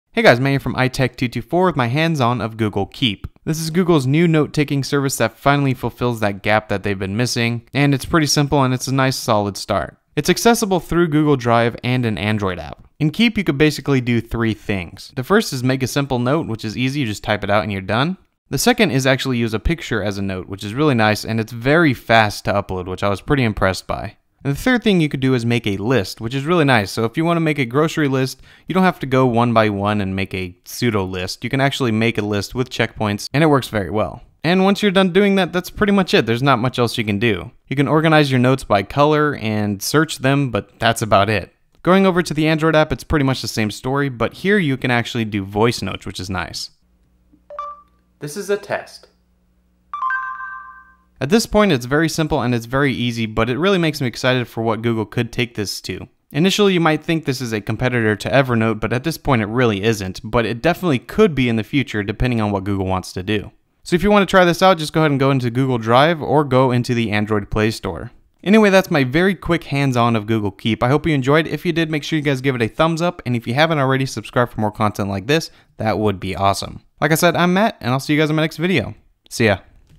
Hands On: Google Keep
After teasing the release of a new note taking service Google finally released Keep.  This is a very simple note taking service that works through Google Drive.  You can upload a picture, make a note, or make a list.  It is very simple.  For now there isn't much to do but there is definitely room to grow.